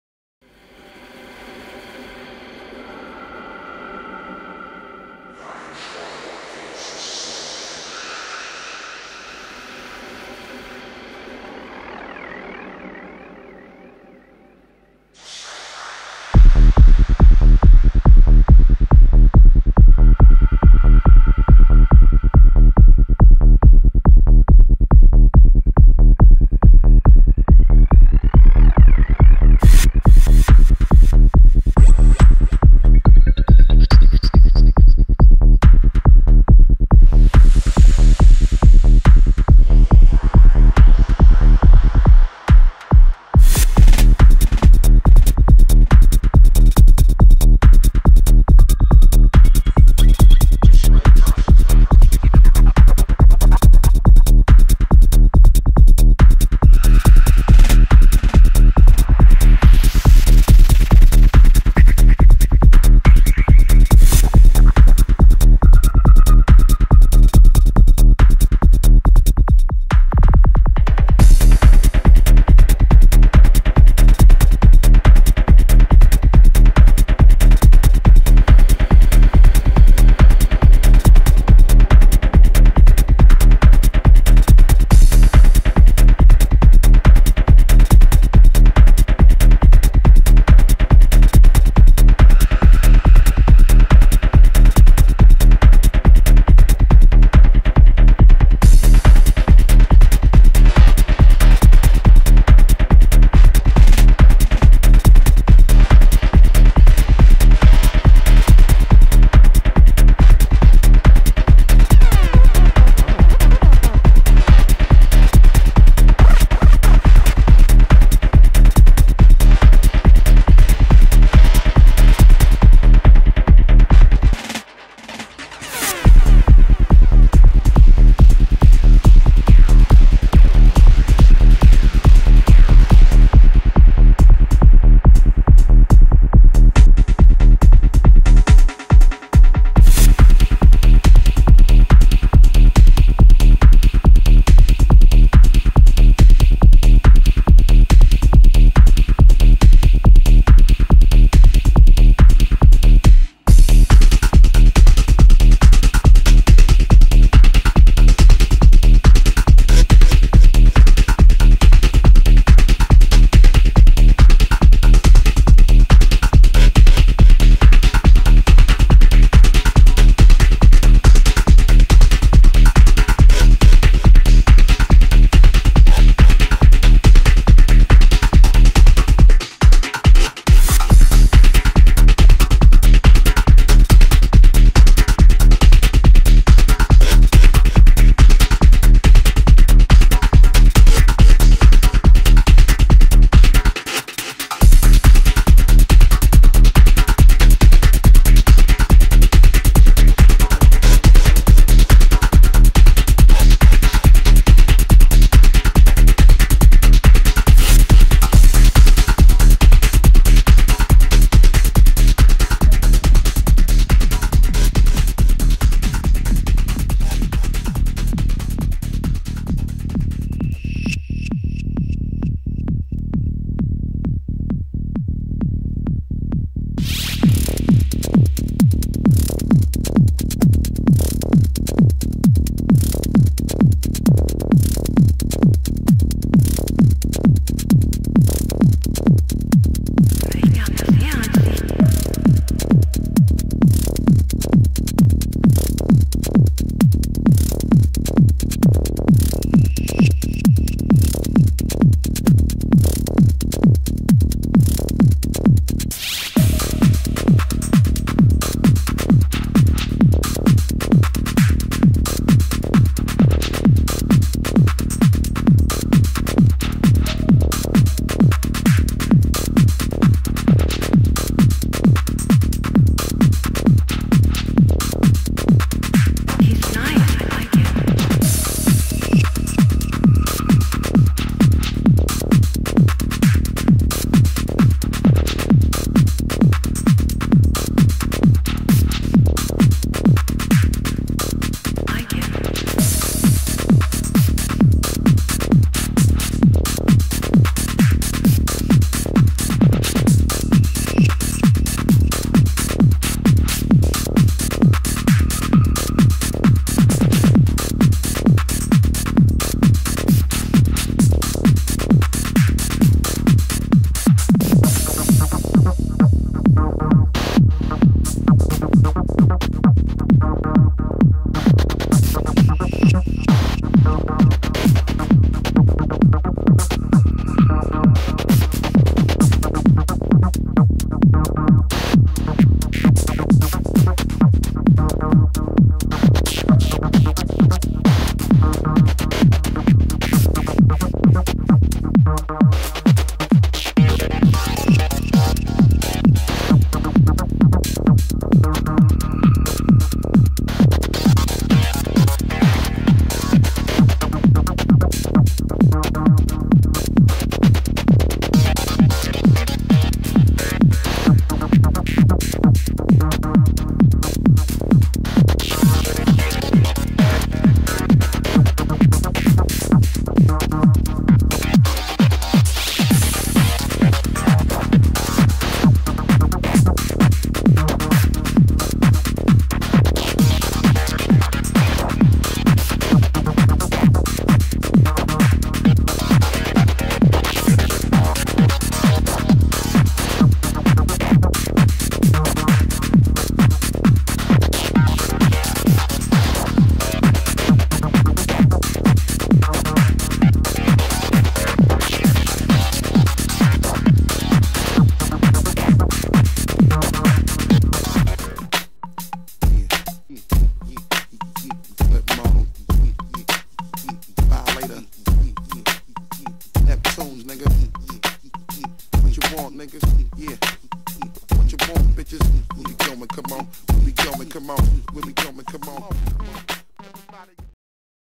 blueraid driving reloaded
driving around in braunau with my girls:
astrid ö. sarah h. sandra k. & marianna z.
Sommer 2008 braunau am inn